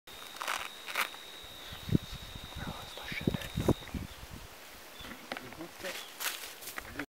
Heloderma charlesbogerti falling out of a tree in Guatemala
This guy was climbing a tree and as we watched it eventually slipped off the branches. This animal was not caught or interfered with in any way at all. It later left and hid.